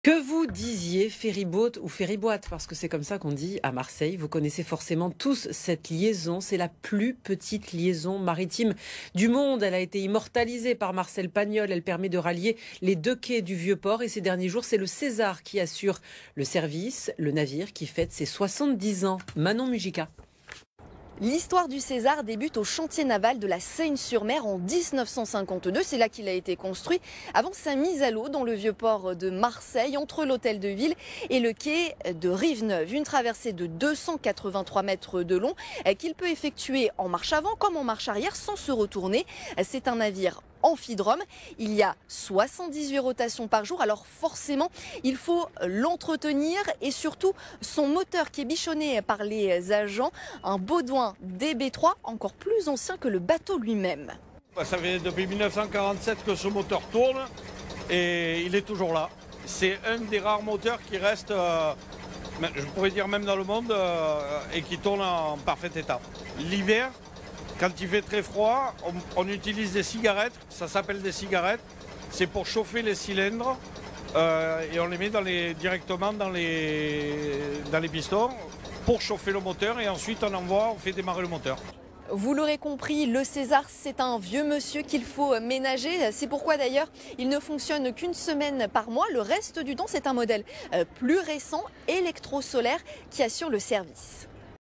Vieux port : le Ferry Boat "César" a 70 ans
C'est la plus petite liaison maritime du monde, le ferry boat permet de rallier les deux quais du Vieux Port. Ces derniers jours, c'est le César qui assure le service, ce navire qui fête ses 70 ans.